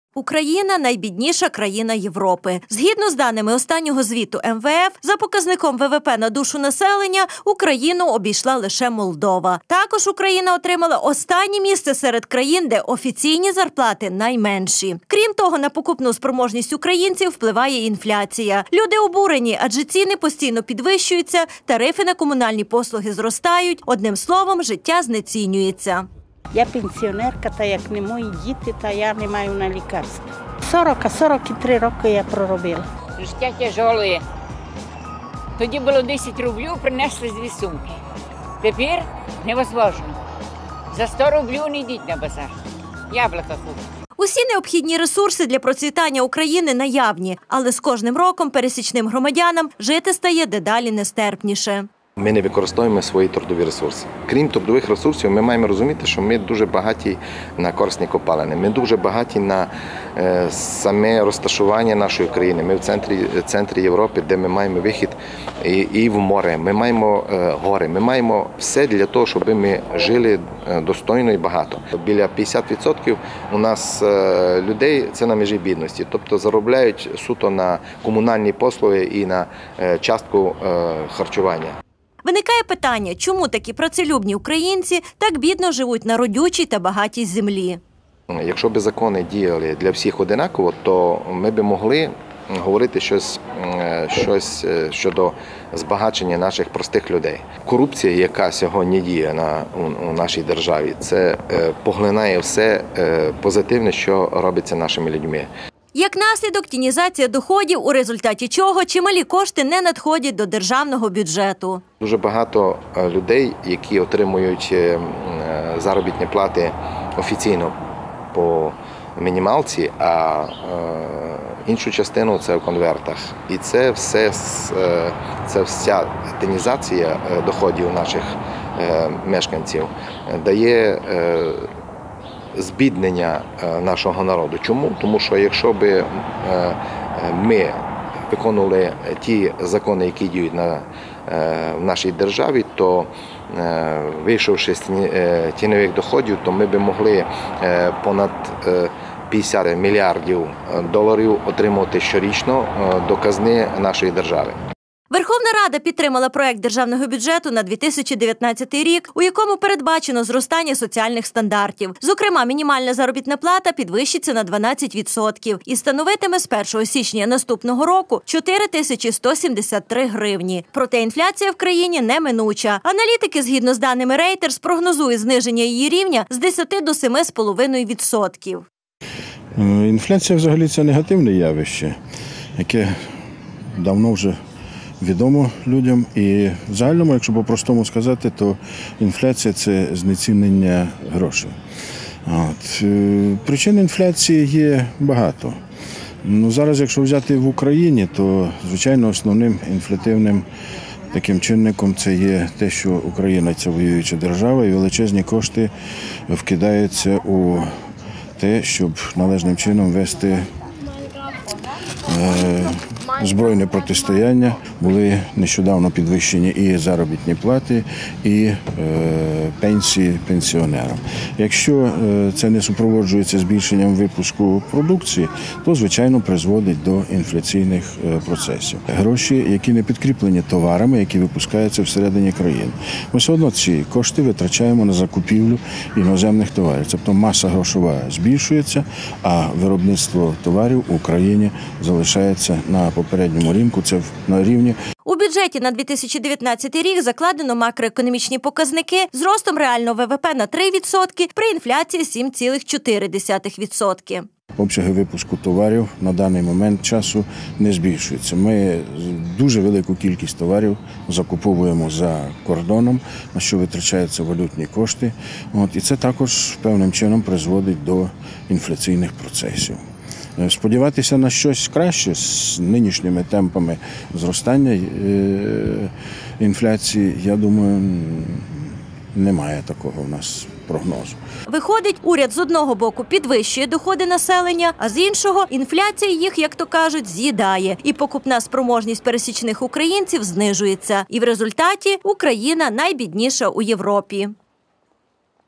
Інфляція та доходи
З 10 до 7,5 % знизиться інфляція в Україні у 2019 році – такі прогнози дають аналітики згідно з даними Рейтерс. Інфляція в країні сповільниться у наступному році до показника 2013 року. Проте Національний банк України у 2018 році анонсував підвищення інфляції з 8,9 до 10,1 %, а прогноз на 2019 рік – 7,4%.